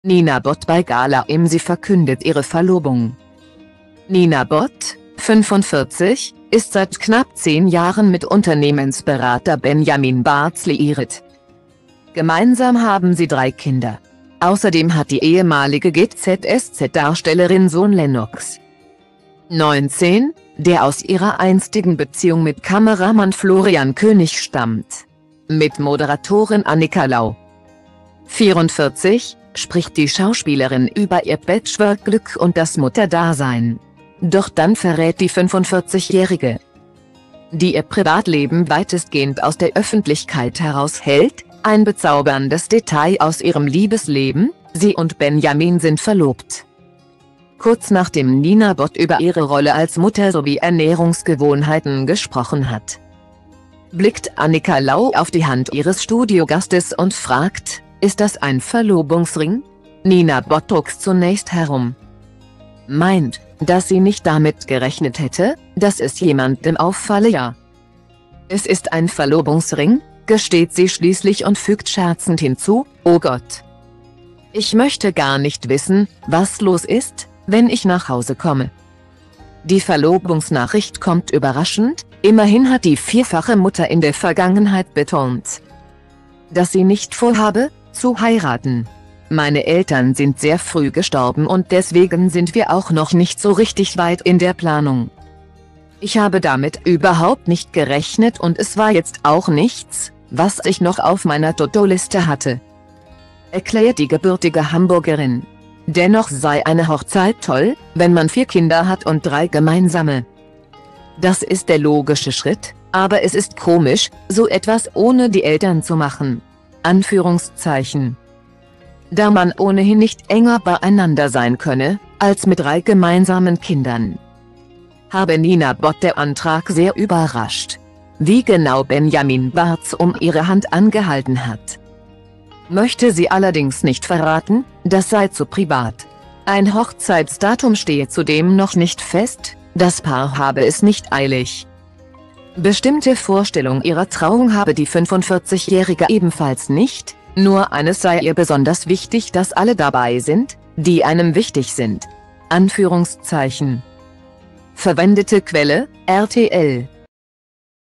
Nina Bott bei GALA im Sie verkündet ihre Verlobung
Nina Bott, 45, ist seit knapp zehn Jahren mit Unternehmensberater Benjamin Baarz liiert,gemeinsam haben sie drei Kinder. Außerdem hat die ehemalige GZSZ-Darstellerin Sohn Lennox,19, der aus ihrer einstigen Beziehung mit Kameramann Florian König stammt. Mit Moderatorin Annika Lau,